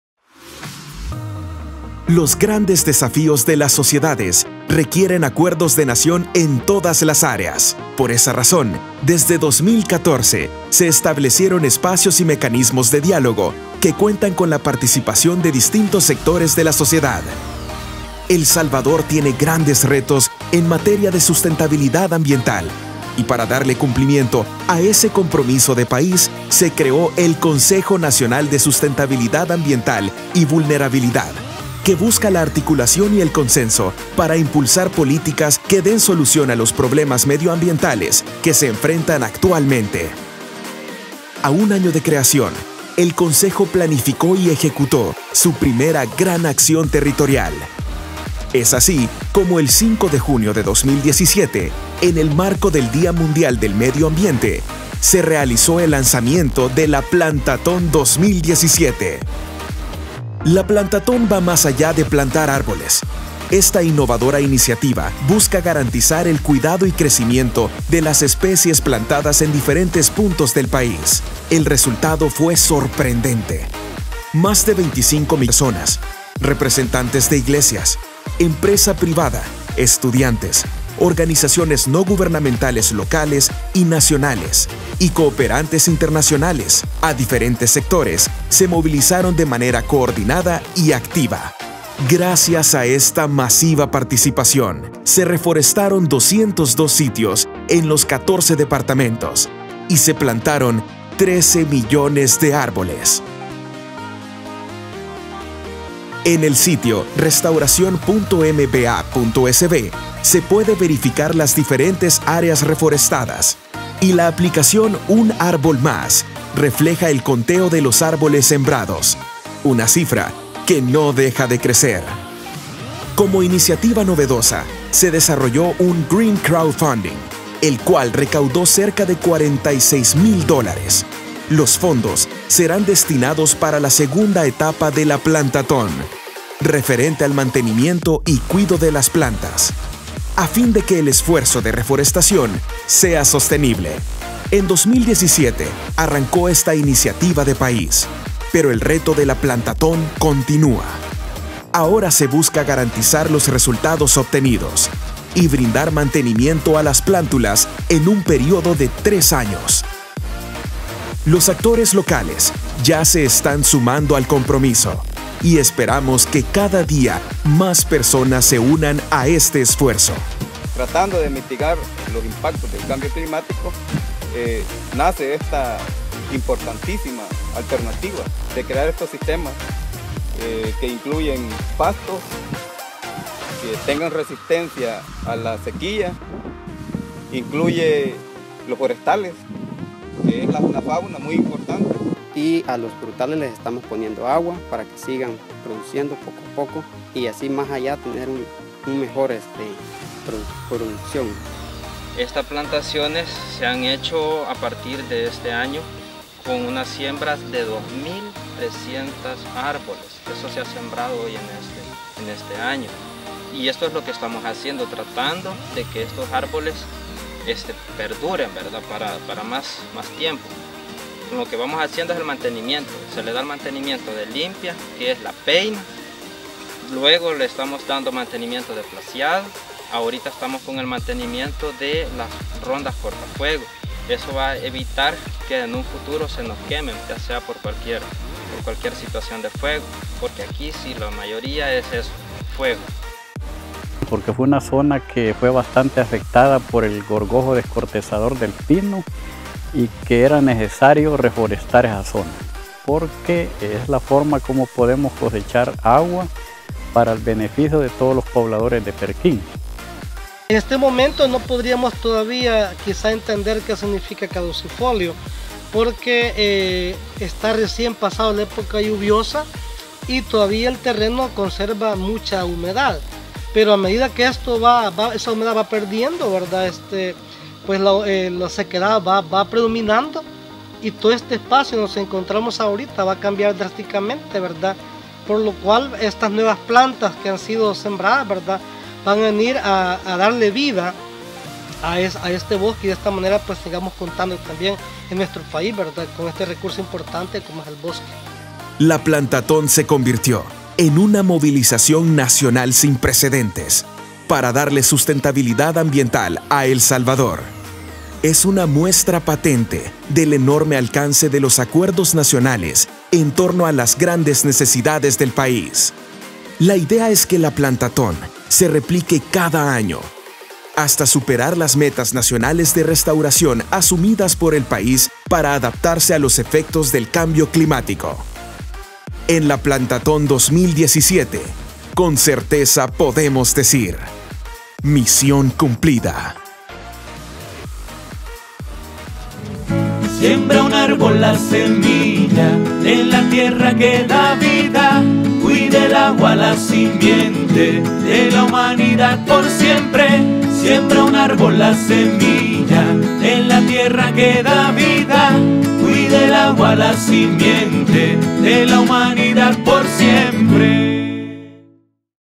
Resultados de la PLANTATÓN 2017
El CONASAV rindió un informe sobre los resultados de la campaña de reforestación masiva PLANTATÓN 2017 y anunció las acciones que se desarrollarán en los próximos meses, para dar mantenimiento y protección a las zonas reforestadas en los 14 departamentos del país.